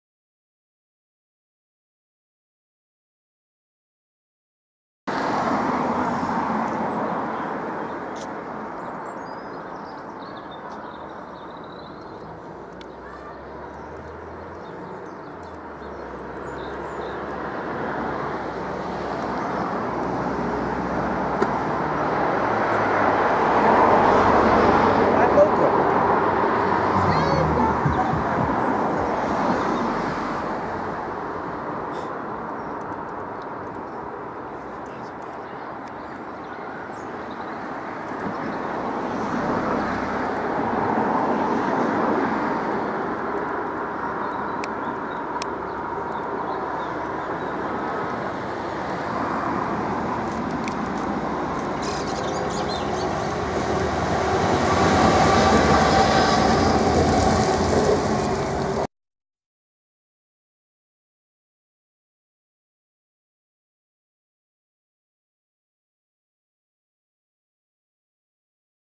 DB Cargo Class 66 No. 66134 on 0V85 Milford West Sidings - Appleford Sidings on 28.02.21 - HD
DB Cargo Class 66 No. 66134 is seen here passing under Stamford Road working the 0V85 Milford West Sidings - Appleford Sidings on the 28th February 2021.